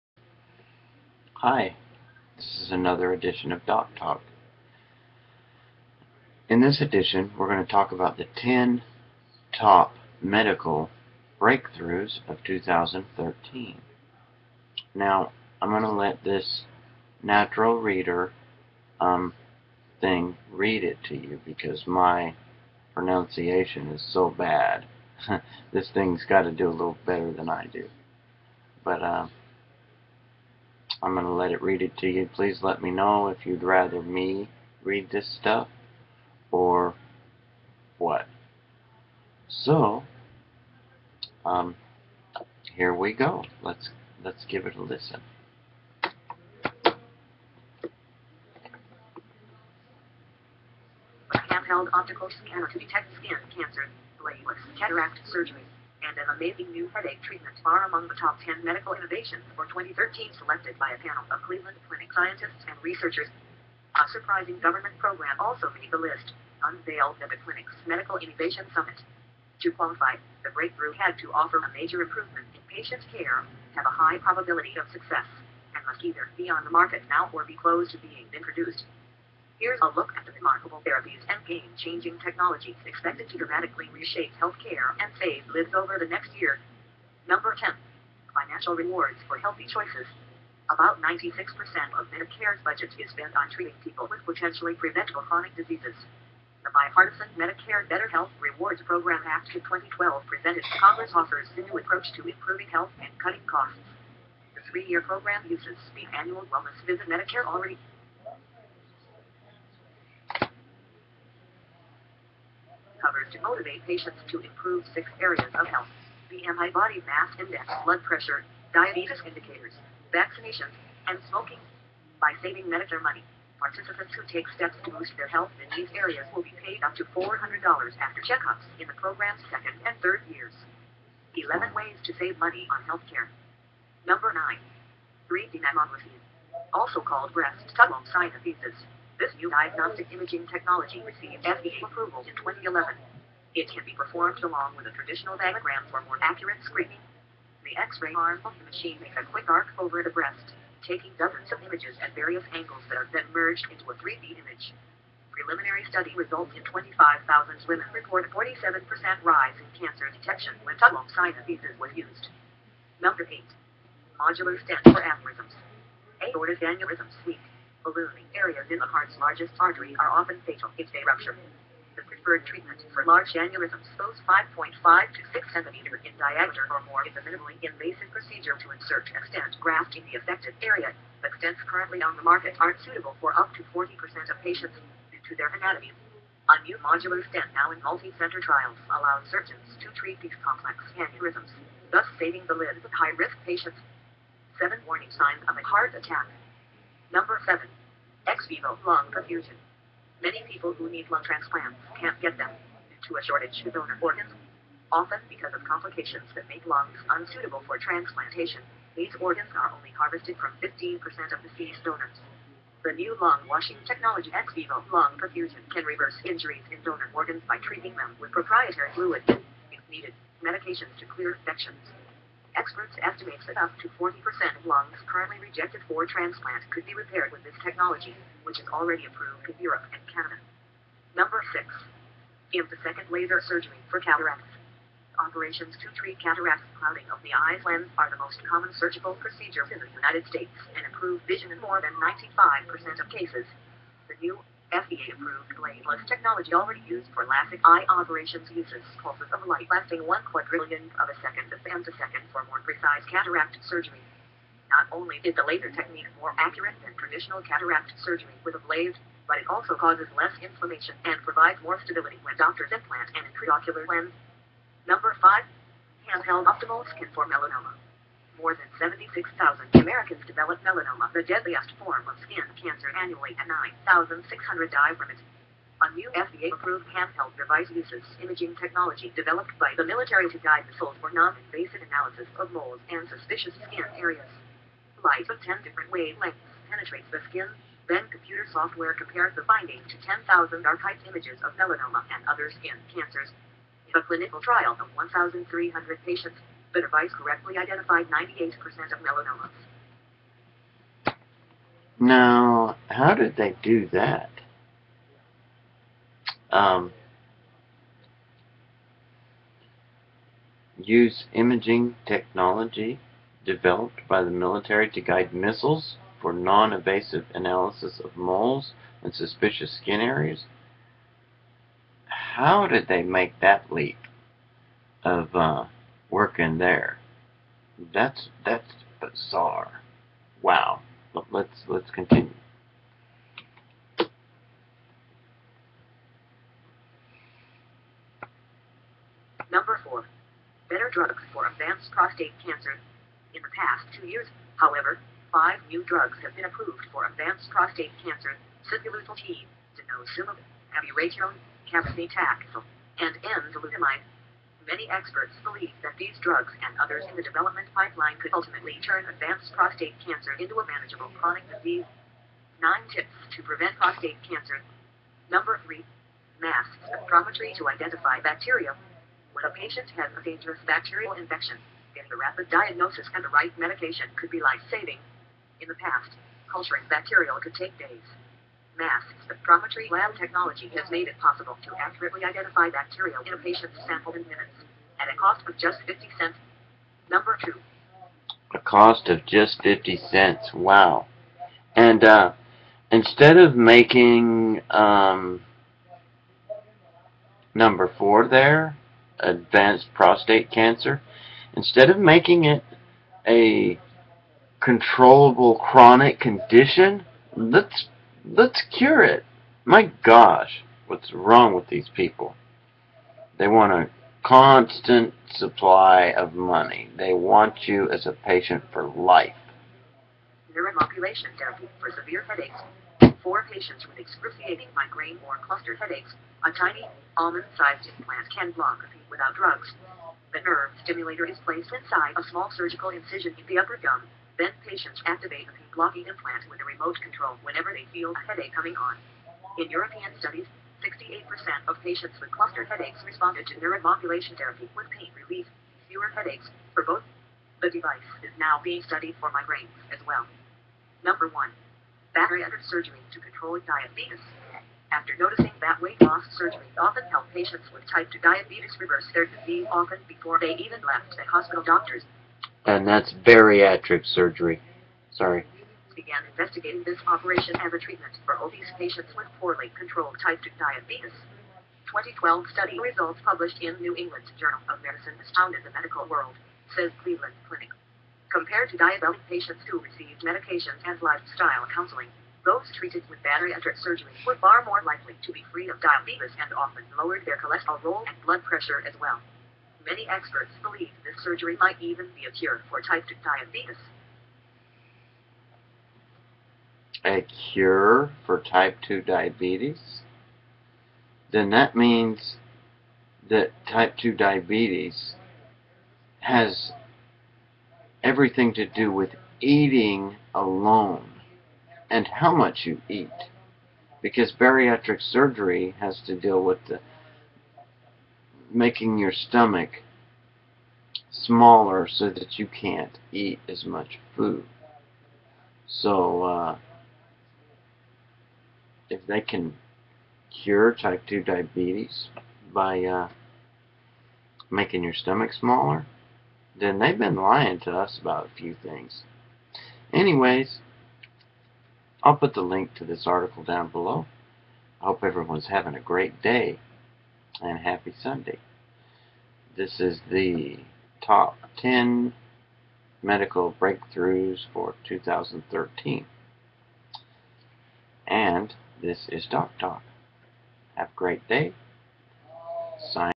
TOP 10 MEDICAL BREAKTHROUGHS FOR 2013..DOCTALK/
ON THIS EPISODE OF DocTalk wee look at the top 10 breakthroughs of 2013.Info from Yahoo health. YouTube Video Editor